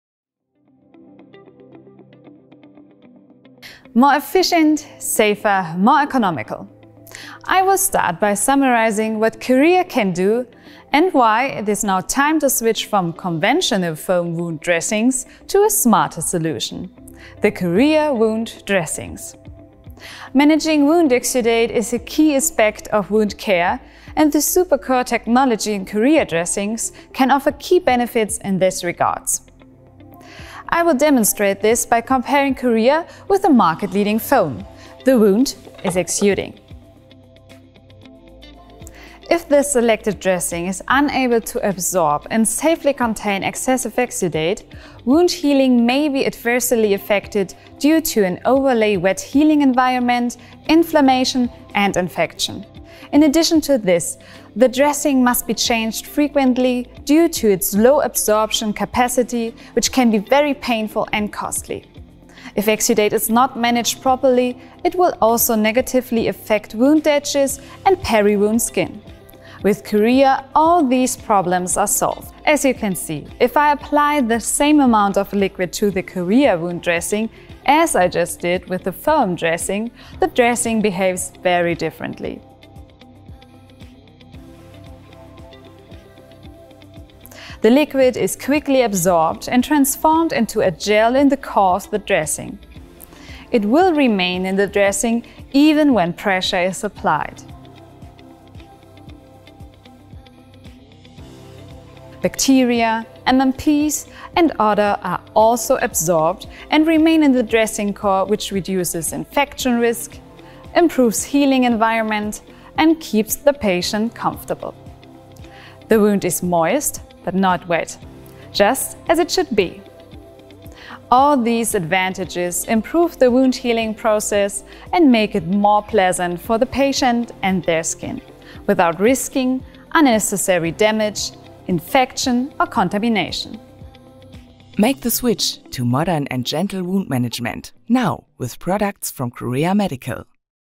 Key benefits of curea dressings against a market leading foam dressing
Managing wound exudate is a key aspect of wound care and the SuperCore®-technology in curea dressings can offer key benefits in this regards.